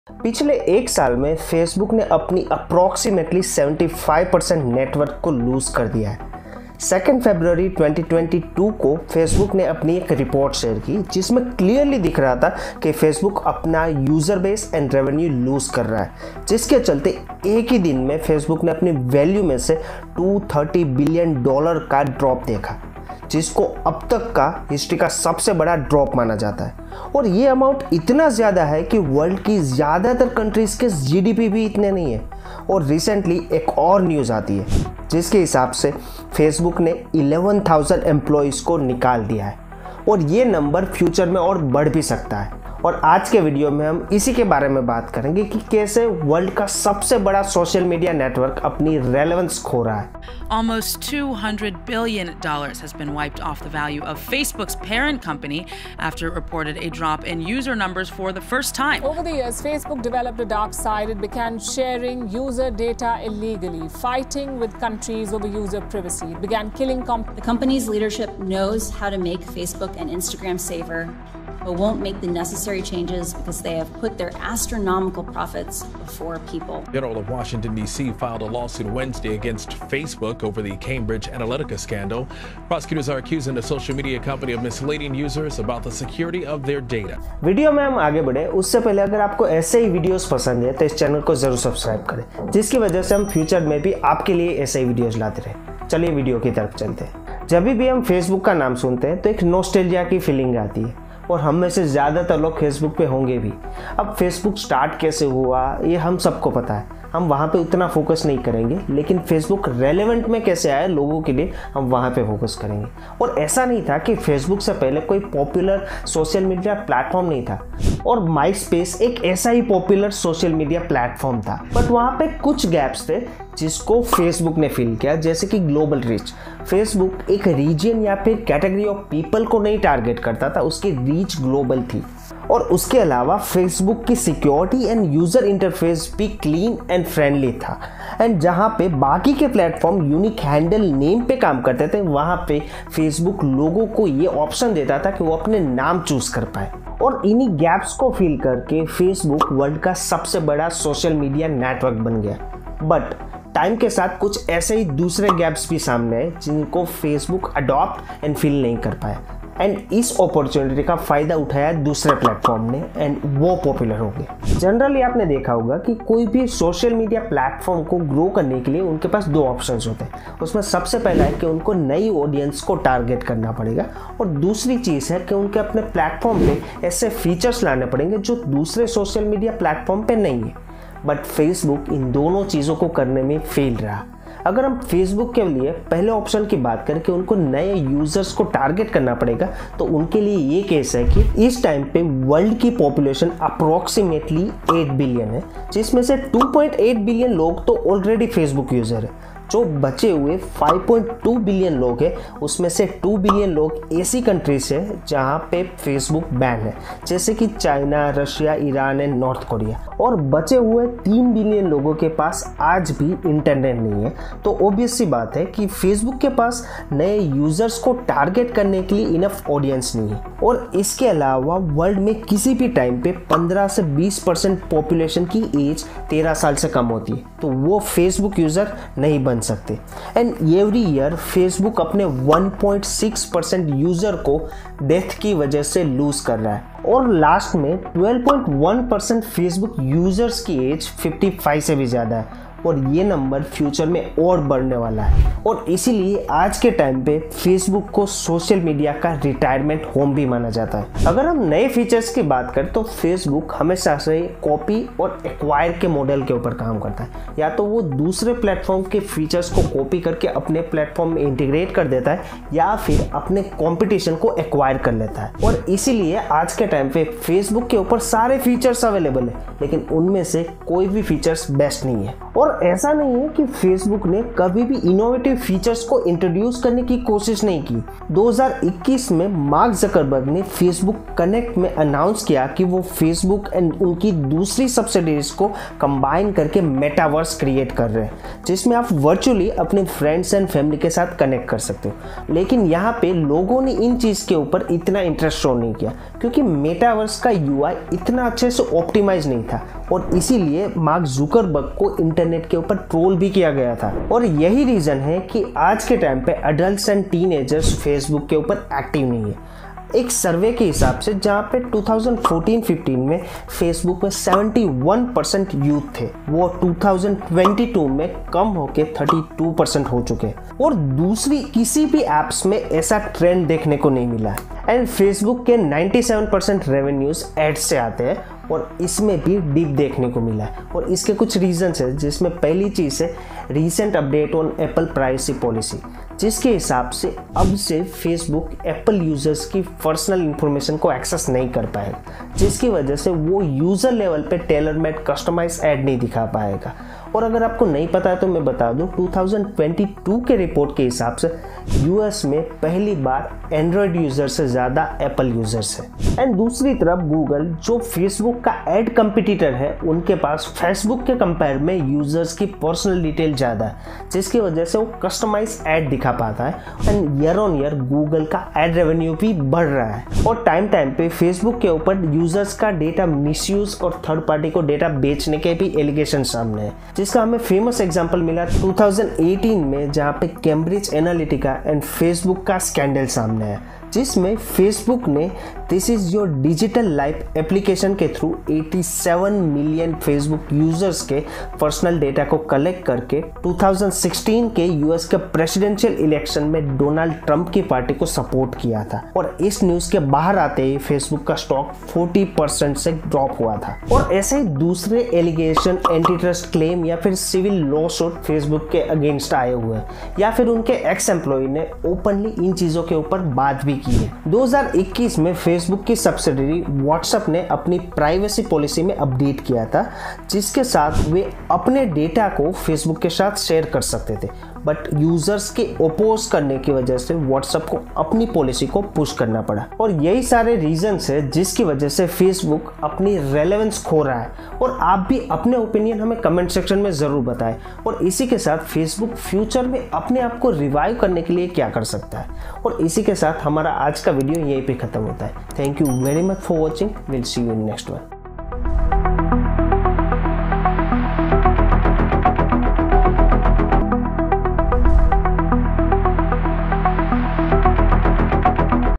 Is Facebook (Meta) Dying? Here's why - Business Case Study | फेसबुक बंद होने वाला है?
In this video, we have discussed about rise and fall of Facebook(Meta), why Facebook is loosing users, teens are not active on Facebook, competition is killing Facebook and claims and data privacy allegation against Facebook which draw to death. watch for more

All CLIPS ARE BELONGS TO RESPECTED OWNERS